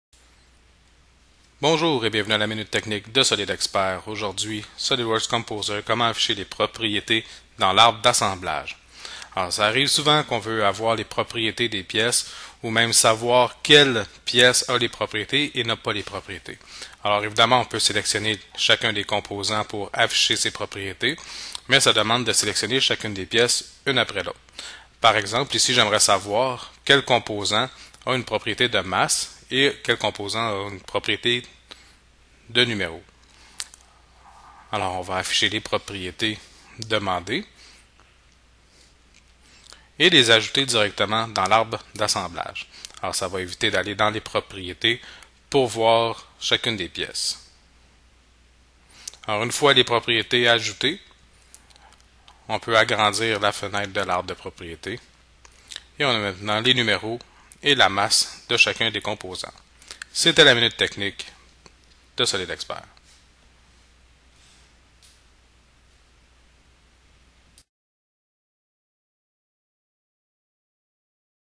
Propriété dans l’arbre d'assemblage - SOLIDWORKS Composer - Juillet 2015
Saviez-vous que les propriétés de SOLIDWORKS sont aussi disponible dans Composer ? Mais comment faire pour les voir dans une colonne? Il est possible d’ajouter une colonne dans l’arbre de création et ainsi voir les propriété des pièces d’un seul coup. Il suffit de connaitre le truc.